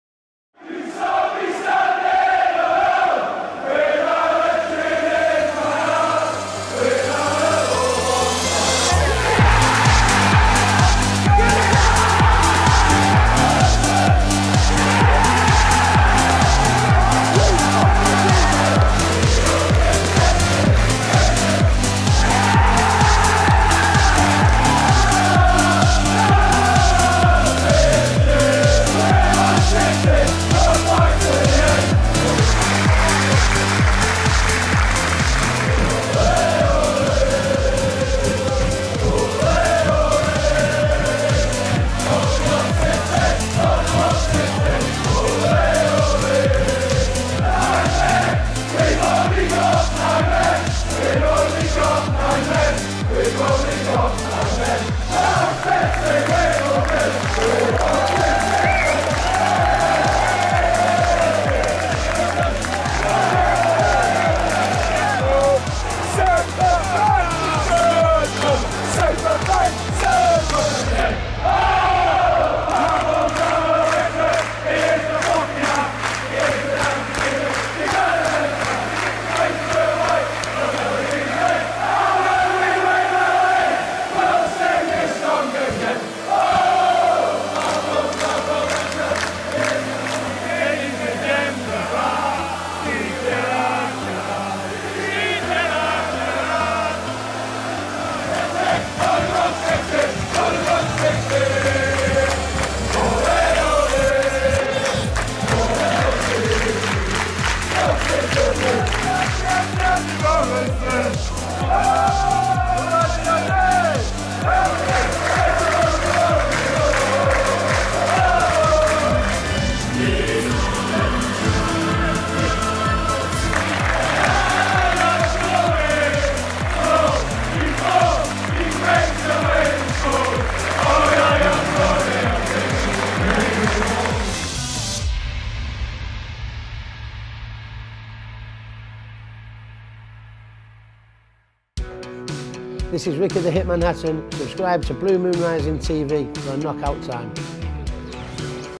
Manchester City Fans React to 2014/15 Season | Chants, Songs, Celebrations
Here's how YOU saw the 2014/2015 season - the best videos recorded by Manchester City fans, with chants, songs and cheers!